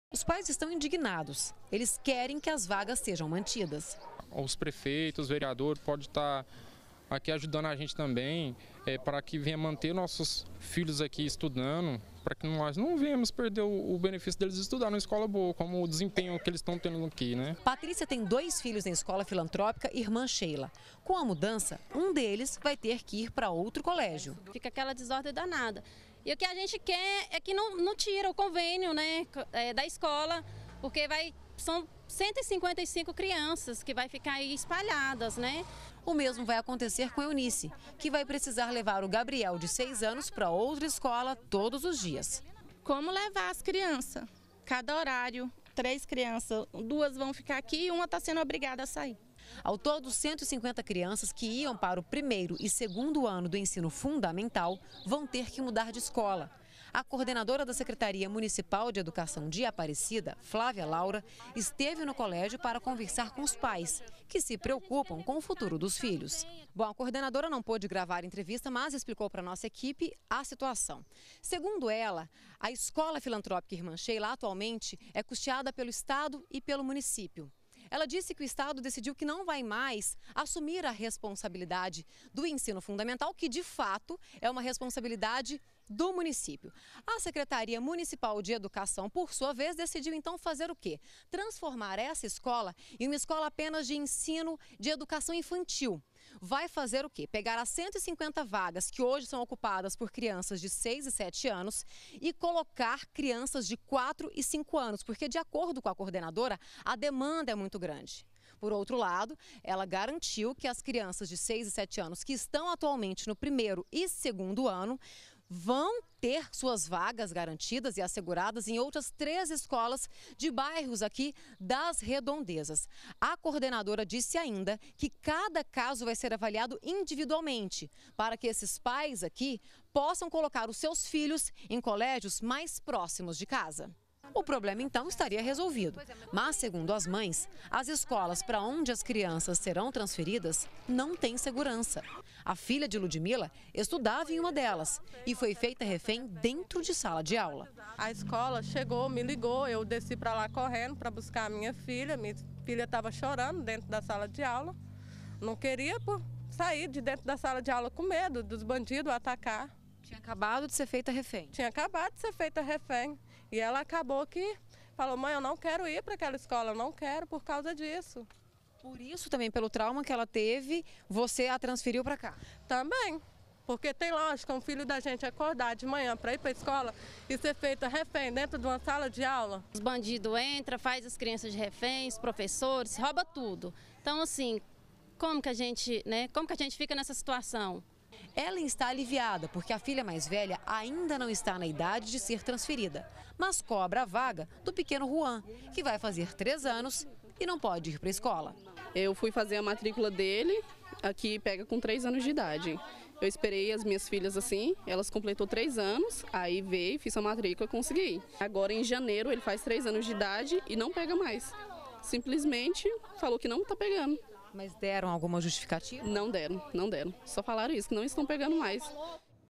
BG - Pais protestam contra saída de alunos - 05-12-2017
Pais de alunos da escola filantrópica Irmã Sheila, de Aparecida de Goiânia fizeram protesto contra a retirada de duas séries. Com a medida, pouco mais de 150 alunos vão ser transferidos para outros colégios da cidade. Mas os pais dos alunos não estão nem um pouco contentes com a mudança.